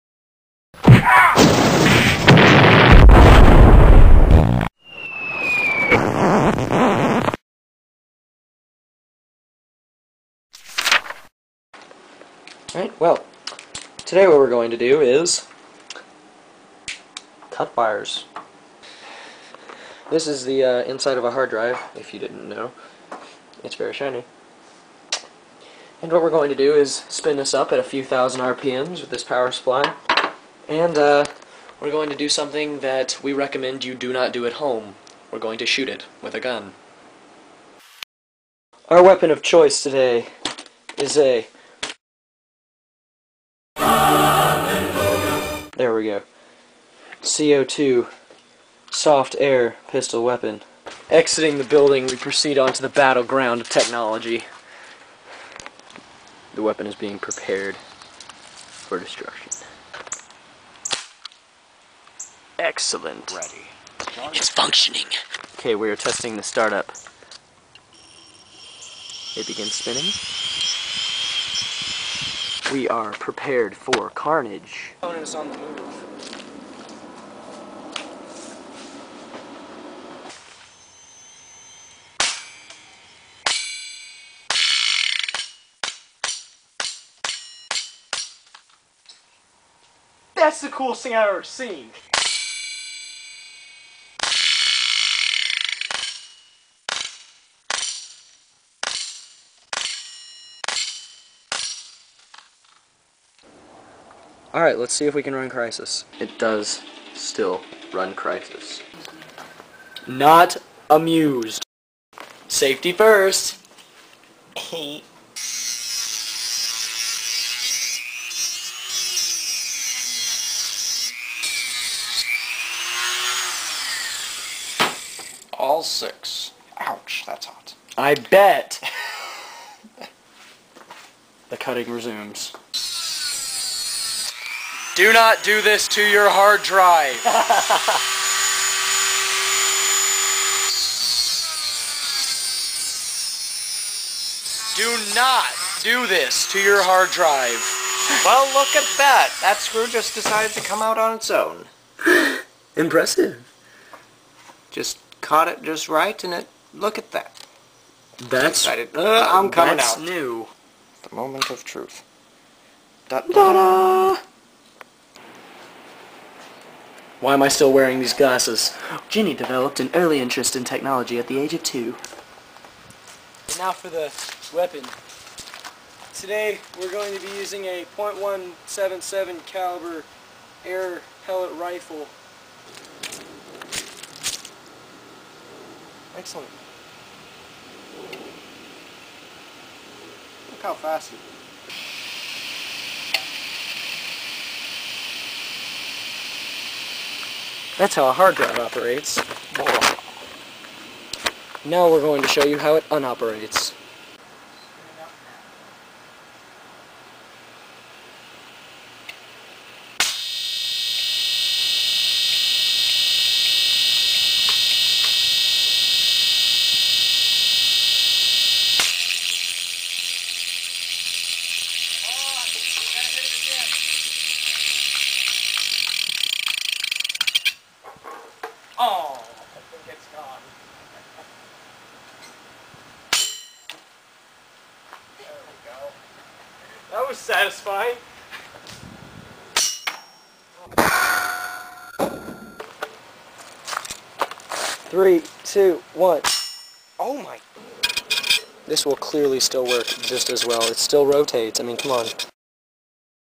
Gun Vs HDD
Firing various low-power guns at two open (and running) hard drives.  Just to see what happens.